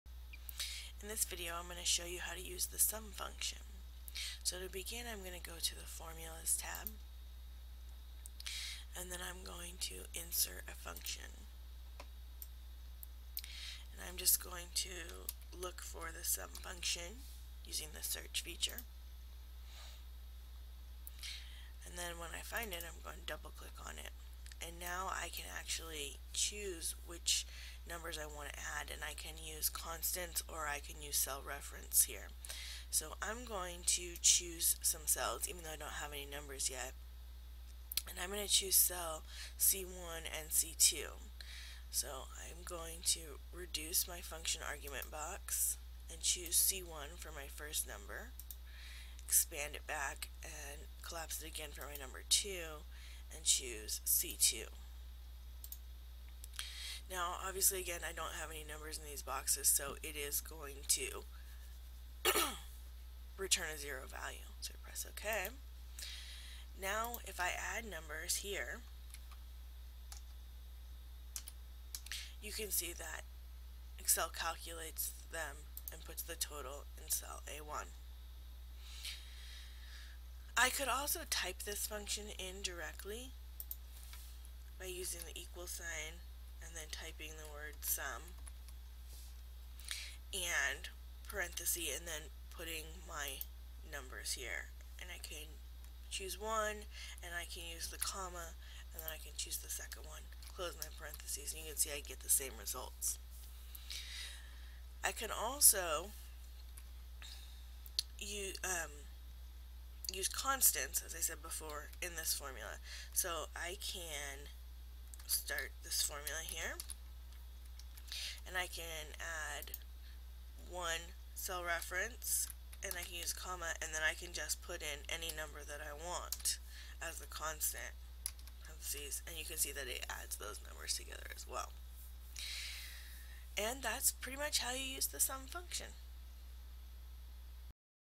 How to Use the SUM function
This video will show you the basics for using the SUM function in Excel.